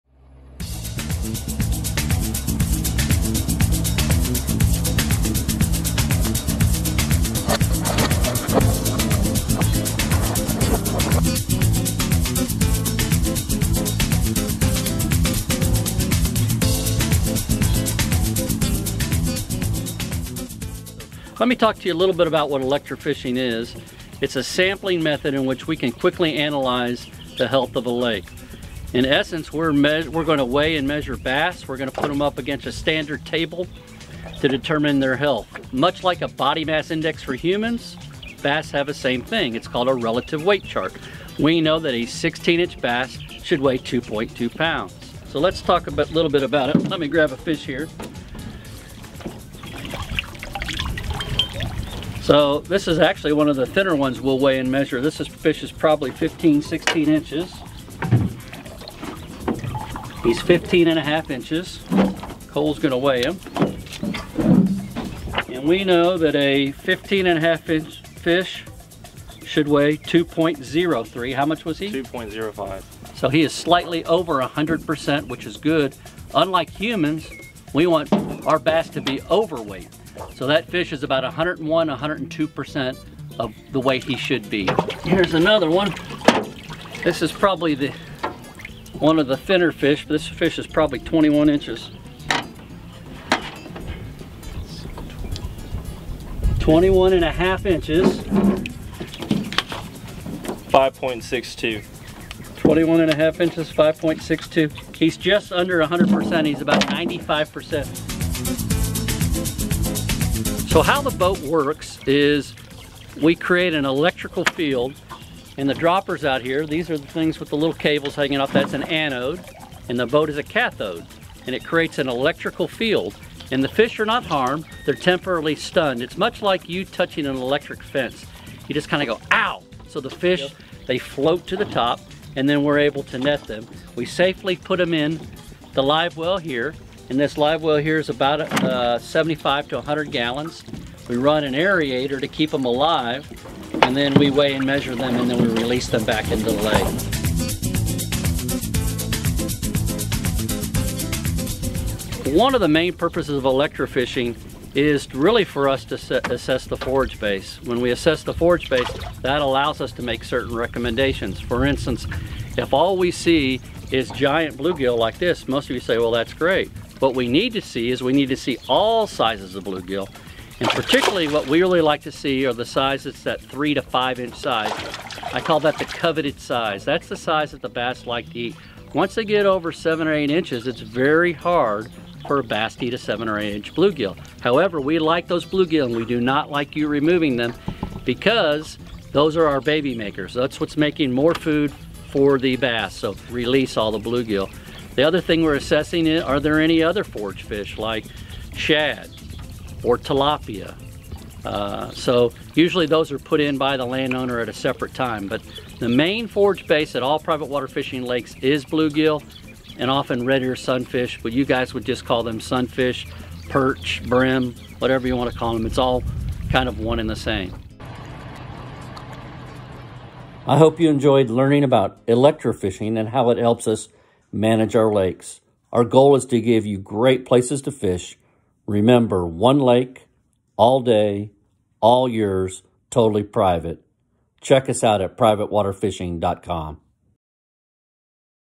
ELECTROFISHING - What Why How? The owner of Private Water Fishing explains Electrofishing
What is electrofishing, why and how do we do it? Private Water Fishing club owner Steve Alexander explains how it is used to enhance bass fishing in our club lakes.